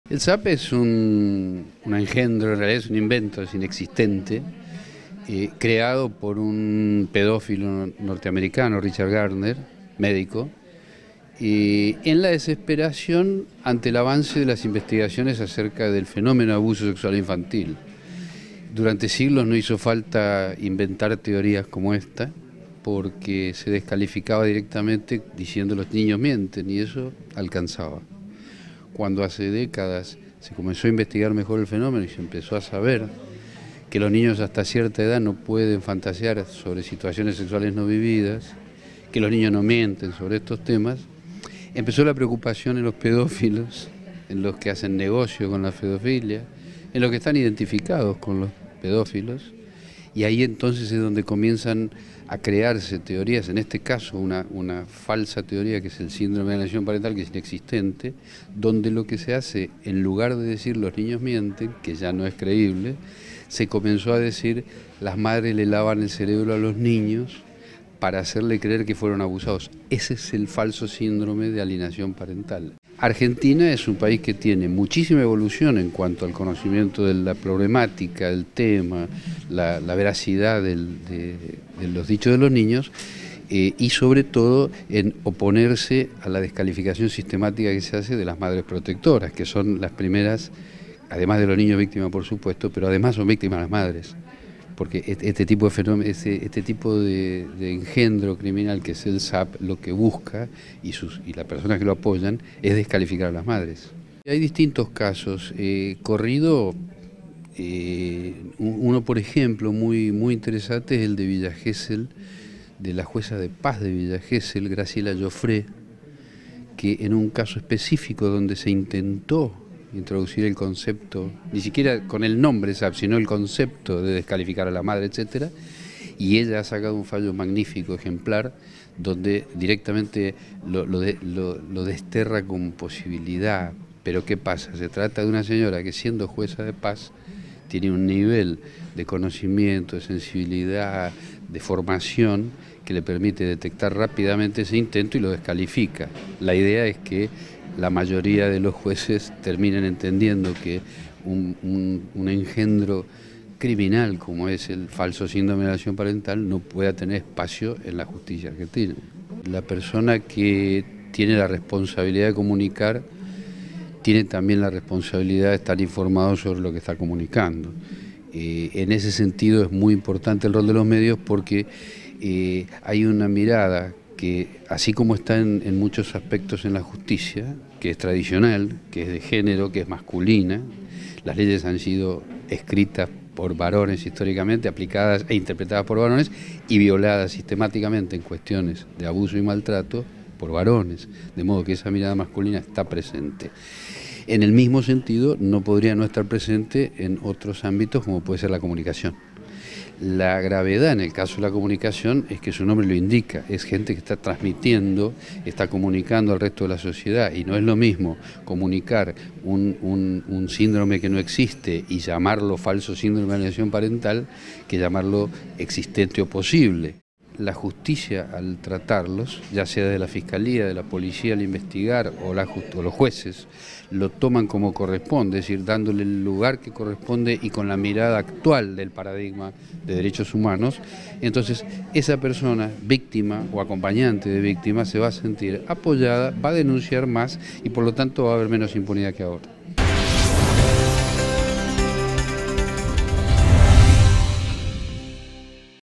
Previenen sobre teorías infundadas utilizadas para descalificar a las madres
Para el juez federal de La Plata Carlos Rozanski, alertó que el denominado "síndrome de alienación parental" es inexistente científicamente y genera confusión

El magistrado autor del libro "Abuso sexual infantil, ¿denunciar o silenciar?" y redactor del texto de la ley 25.852 (que protege derechos de niños o niñas víctimas), considera que es un modo de descalificar a las madres, describiéndolas y ubicándolas como"manipuladoras" y "locas".

"Simplemente es una parte más de la reacción contra el avance que hubo en abuso sexual infantil", interpretó Rozanski en declaraciones a Télam.

"La Argentina -destacó el juez- ha tenido avances muy importantes en lo que respecta al estudio de lo relacionado con el abuso sexual, pero aun así los medios de comunicación tradicionales presentan en muchas oportunidades una vulneración a la hora de comunicar estos temas".

"No sólo por condenar a las víctimas, sino que en muchas oportunidades no conocen sobre el tema o están desinformados de la situación particular de la que están hablando", agregó.

Según Rozanski, "en realidad, si nos remontamos al origen de las primeras nominaciones del síndrome de alineación parental, durante siglos se dijo que los chicos mienten, y así se resolvían los problemas sobre maltratos en la infancia en instancias judiciales".

El juez explicó que "cuando se supo que los chicos no mienten, sobre todo frente al relato que ponía en evidencia las situaciones de abusos sexuales, se empezó a avanzar en la protección en la infancia, y aquí los pedófilos o violentos tuvieron varios problemas en su defensa".

"Después vino este síndrome, importado de un autor americano que ni siquiera estaba convalidado en su país", señaló Rozanski.

Este diagnóstico ubicaba a los niños frente a los adultos, y sostenía que se les había lavado el cerebro para que denunciaran que habían sido abusados.

El juez afirmó que "gracias a los avances en las luchas sobre derechos humanos y a las leyes de protección, entre otras acciones, en la Argentina este concepto se está desestimando".

De todos modos, advirtió el magistrado, "algunos sectores más hegemónicos de la justicia o de los medios de comunicación persisten en teñir sus discursos y acciones históricamente prejuiciosos por género o edad".